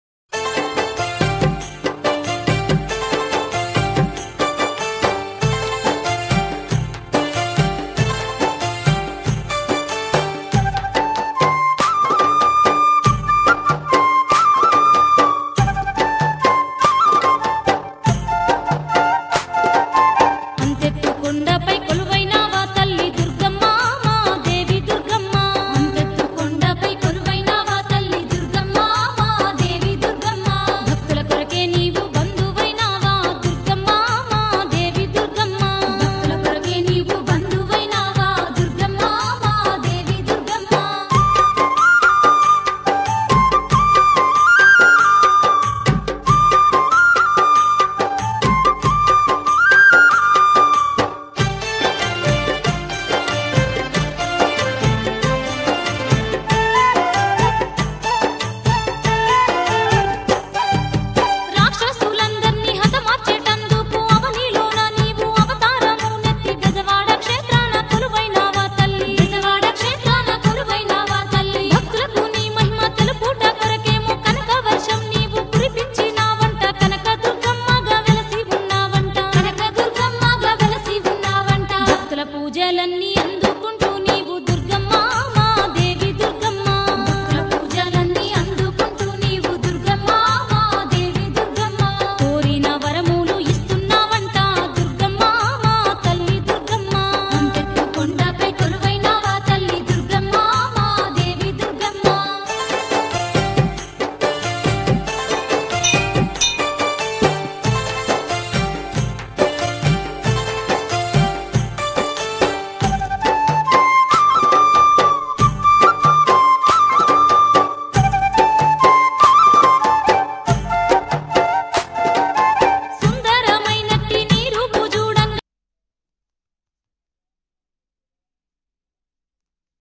friends శ్రీ దుర్గా భైరవకోన కొండ ! శ్రీ దుర్గా భవాని మాత చూడదగ్గ ఆలయం ! జై దుర్గా జై జై దుర్గ 🙏!!
🙏నెల్లిమర్ల మండలం రామ తీర్థాలు సమీపంలో గల గొర్లిపేట గ్రామంలో ఉన్న ప్రసిద్ధిగాంచిన శ్రీ దుర్గా భైరవకోన కొండ శ్రీ దుర్గా భవాని మాత చూడదగ్గ ఆలయం
జై దుర్గా భవాని
జై జై దుర్గ భవాని 🙏🙏🙏